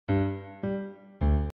In those with learning issues on developmental issues...
In  those with learning issues on developmental issues, very often there's lack of connections between vestibular, the balance organ, and the cerebellum and as a result, the cerebellum isn't powered up as much, it isn't able to do its job as much.

What’s your take on this?